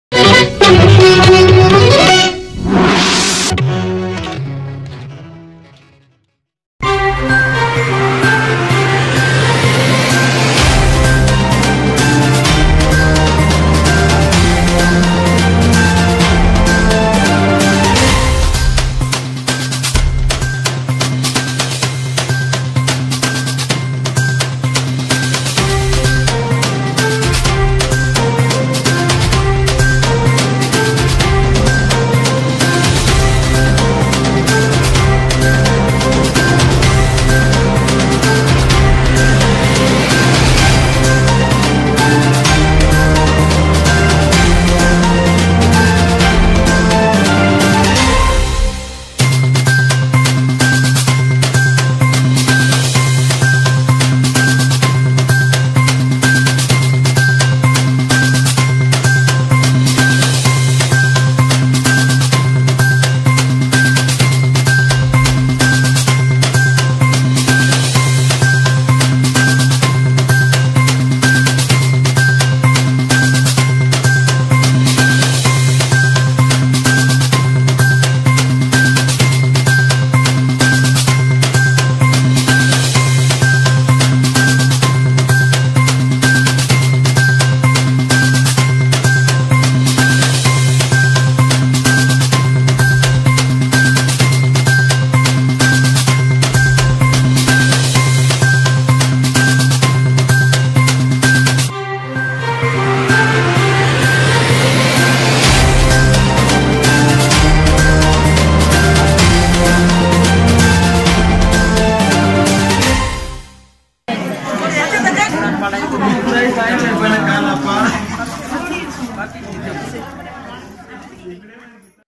🔥వికారాబాద్ జిల్లా కొడంగల్ నియోజకవర్గం బొంరాస్పేట్ మండల కేంద్రంలో ప్రశాంతంగా ఎన్న
🔥వికారాబాద్ జిల్లా కొడంగల్ నియోజకవర్గం  బొంరాస్పేట్ మండల కేంద్రంలో ప్రశాంతంగా ఎన్నికలు జరిగాయి 🔥